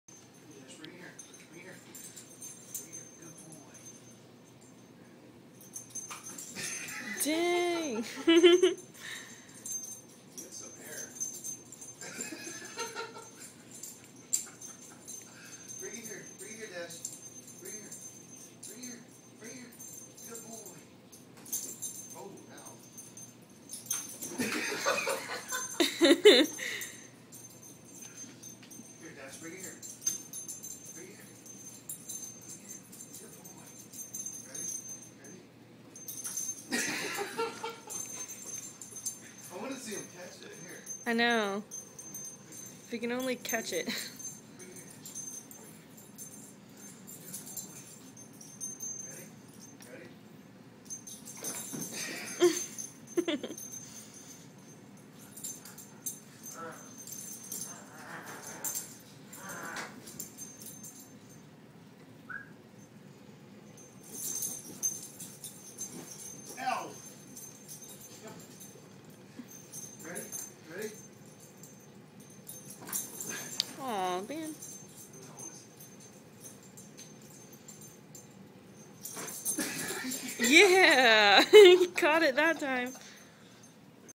Slick the amazing flying border collie
Slick and dash goin hard at the frisbee